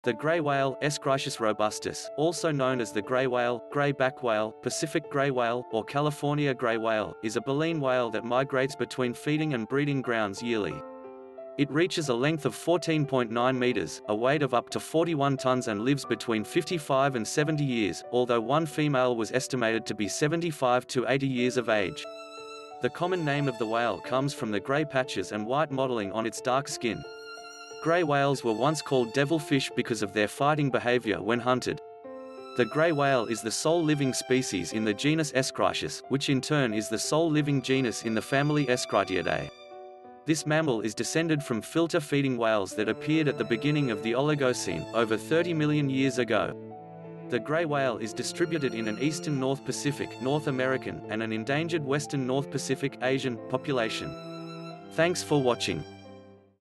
Gray Whale || Description and Facts!
The gray whale (Eschrichtius robustus), also known as the grey whale, gray back whale, Pacific gray whale, or California gray whale, is a baleen whale that migrates between feeding and breeding grounds yearly. It reaches a length of 14.9 meters (49 ft), a weight of up to 41 tonnes (90,000 lb) and lives between 55 and 70 years, although one female was estimated to be 75–80 years of age. The common name of the whale comes from the gray patches and white mottling on its dark skin. Gray whales were once called devil fish because of their fighting behavior when hunted. The gray whale is the sole living species in the genus Eschrichtius, which in turn is the sole living genus in the family Eschrichtiidae. This mammal is descended from filter-feeding whales that appeared at the beginning of the Oligocene, over 30 million years ago.

The gray whale is distributed in an eastern North Pacific (North American), and an endangered western North Pacific (Asian), population. North Atlantic populations were extirpated (perhaps by whaling) on the European coast before AD 500, and on the American coast around the late 17th to early 18th centuries. Even so, on May 8, 2010, a sighting of a gray whale was confirmed off the coast of Israel in the Mediterranean Sea,  leading some scientists to think they might be repopulating old breeding grounds that have not been visited for centuries. In May and June 2013, a gray whale was sighted off the coast of Namibia – the first confirmed in the Southern Hemisphere. The round-trip journey of one gray whale has set a new record for the longest mammal migration, covering a distance of more than 22,000 kilometres across the Pacific Ocean. Her migration has shown new insight into how endangered species are making drastic changes in their life style.